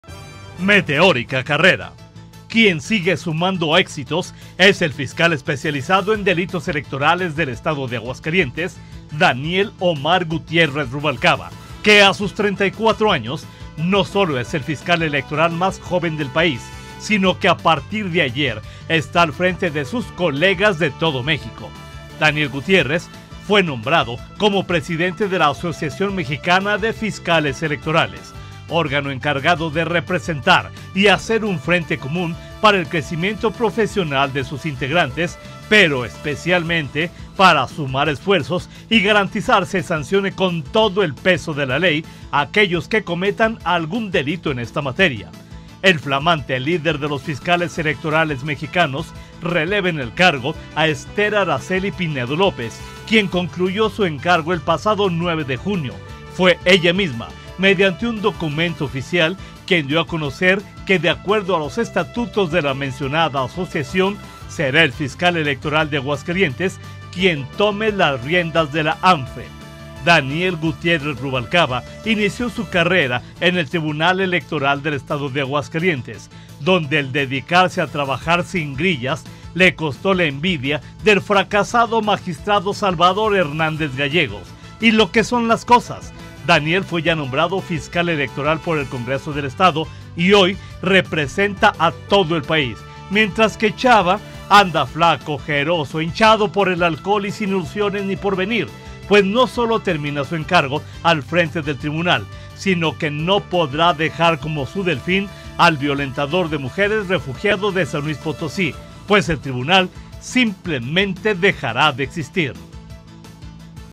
Crónica del Poder/Meteórica carrera
Meteórica carrera

Aguascalientes, Ags, 19 de junio 2024.- Quien sigue sumando éxitos es el Fiscal Especializado en Delitos Electorales del Estado de Aguascalientes, Daniel Omar Gutiérrez Ruvalcaba, que a sus 34 años no solo es el fiscal electoral más joven del país, sino que a partir de ayer está al frente de sus colegas de todo México.
Daniel Gutiérrez fue nombrado como Presidente de la Asociación Mexicana de Fiscales Electorales, órgano encargado de representar y hacer frente común para el crecimiento profesional de sus integrantes, pero especialmente para garantizar se sancione con todo el peso de la ley a aquellos que cometan algún delito en esta materia.
El flamante líder de los fiscales electorales mexicanos releva en el cargo a Esther Araceli Pinelo López, quien concluyó su encargo el pasado 9 de junio. Fue ella misma quien mediante un documento oficial dio a conocer que, de acuerdo a los estatutos de la mencionada Asociación, será el Fiscal Electoral de Aguascalientes quien tome las riendas de la AMFE.
Daniel Gutiérrez Ruvalcaba inició su carrera en el Tribunal Electoral de Aguascalientes, donde el dedicarse a trabajar sin grillas le costó la envidia del fracasado magistrado Salvador Hernández Gallegos y lo que son las cosas, Daniel fue nombrado Fiscal Electoral por el Congreso del Estado y hoy representa a todo el país, mientras que Salvador anda flaco, ojeroso, hinchado por el alcohol, sin ilusiones ni porvenir, pues no solo termina su encargo al frente del Tribunal sino que no podrá dejar como su delfín al violentador de mujeres refugiado de San Luis Potosí, pues el Tribunal simplemente dejará de existir.